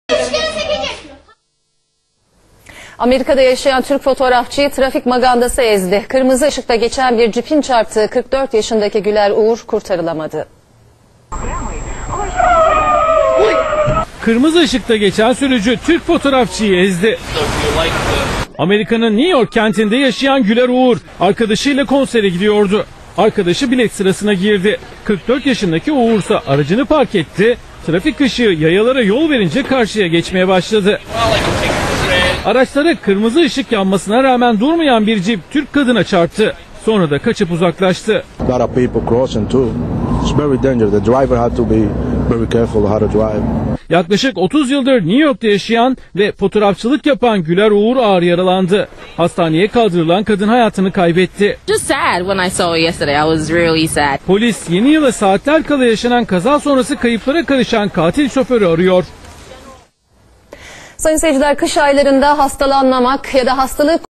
Türk Fotoğrafçı Trafik Kazası Kurbanı
ABD'de kırmızı ışıkta geçen sürücü Türk fotoğrafçıyı ezdi.